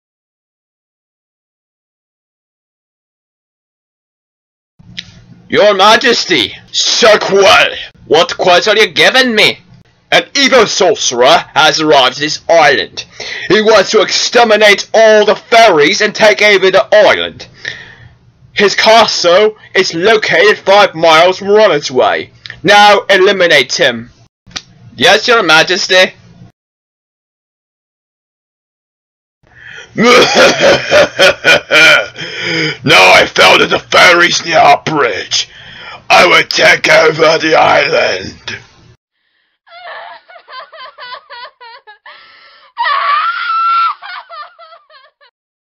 Sneak peak for fantasy movie
I decided to make my upcoming animation as a fantasy genre. Because I want a different topic and a different setting.